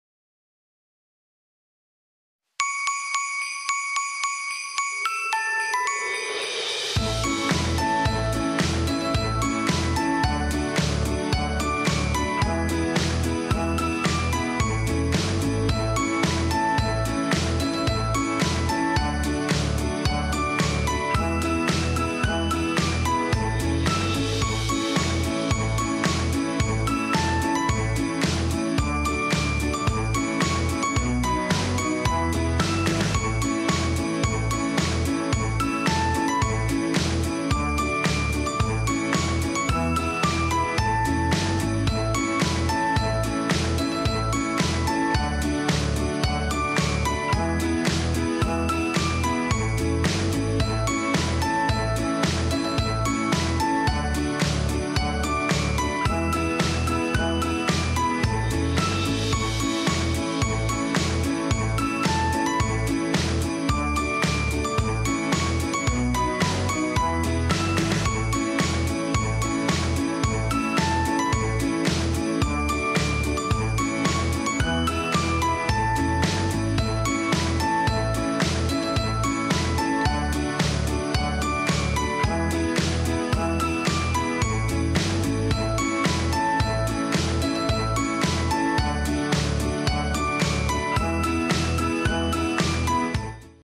2021 HolidayGreetings - G.K. Chesterton, Buddy, Iliana & Nicholas
4-legged friends wish folks Howly Holidays & Meowy New Year.